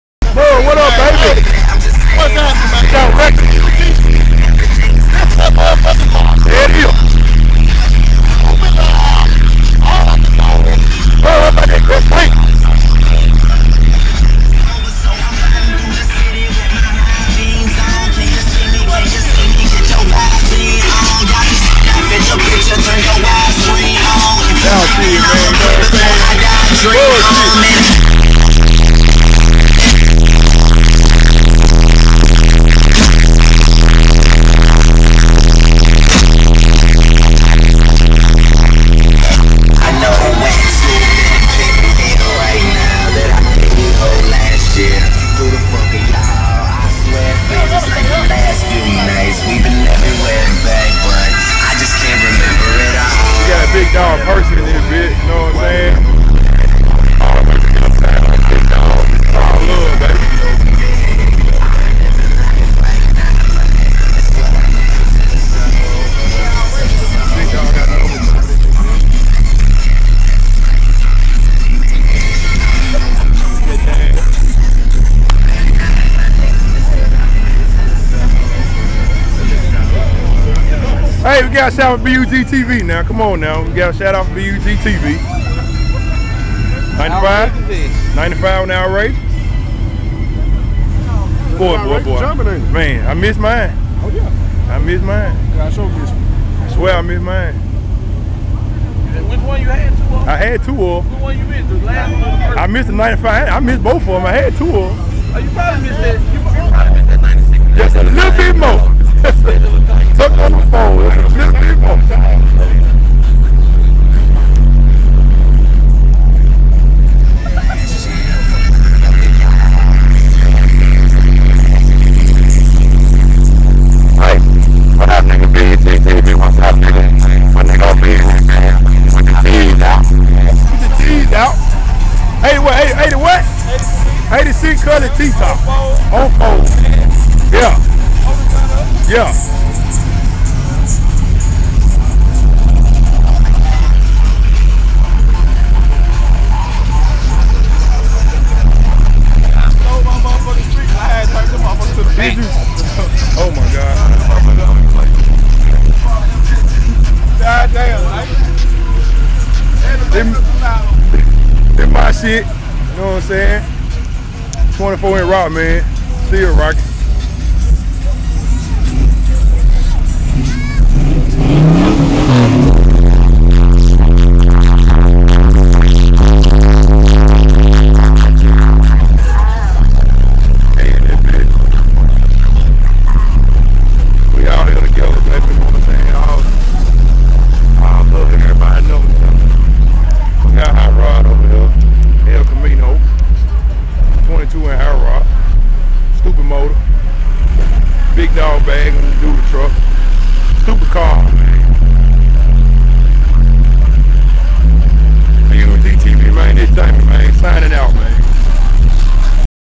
Me, Percy,Jamie,Rodney, Tk,Teddy, killin the parking lot
We went hard after the carshow